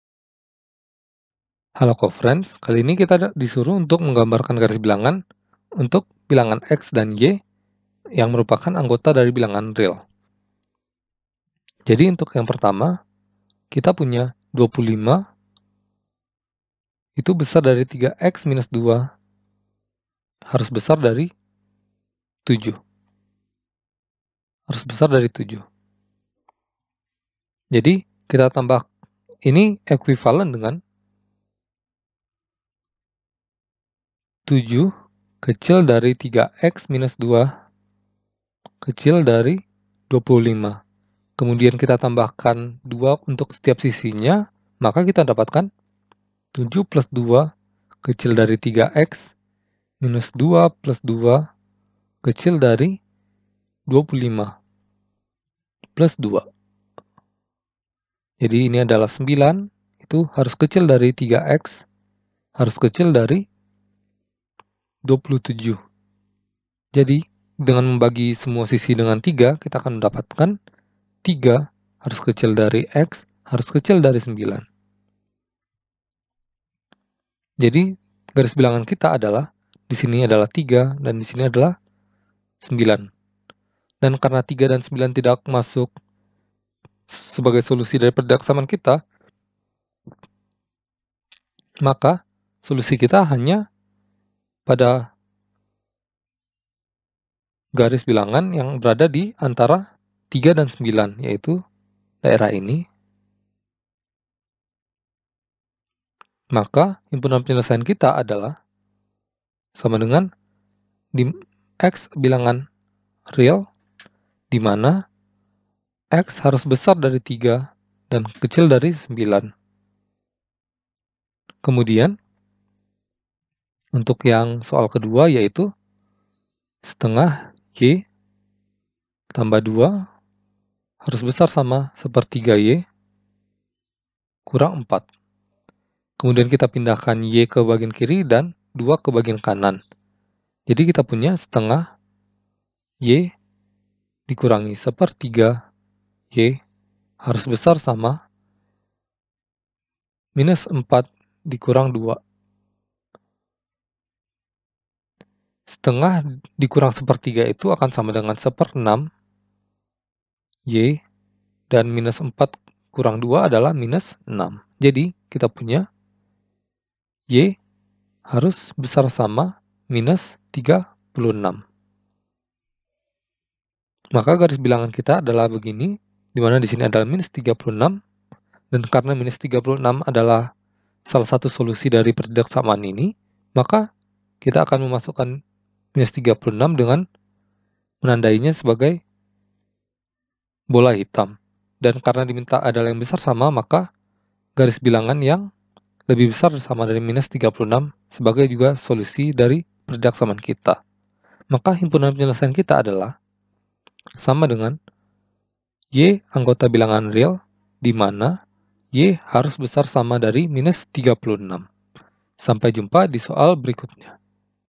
1.25 3x 2 &gt; 7 = Zy + 2 2 1 2_ 4 3Y -
1.25 3x 2 &gt; 7 = Zy + 2 2 1 2_ 4 3Y -

Topik atau Materi: Menyelesaikan Pertidaksamaan Linear Satu Variabel (PtLSV) - PERSAMAAN DAN PERTIDAKSAMAAN LINEAR SATU VARIABEL - ALJABAR - Matematika Kelas 7 SMP - KTSP

Sumber Soal: Matematika Kelas 7, KTSP, JD, 2011 , Depdiknas

00:00 : Bedah Soal
01:00 : Konsep, rumus dan pengertian Menyelesaikan Pertidaksamaan Linear Satu Variabel (PtLSV)
01:18 : Langkah penyelesaian soal
02:34 : Jawaban akhir
03:04 : Penutup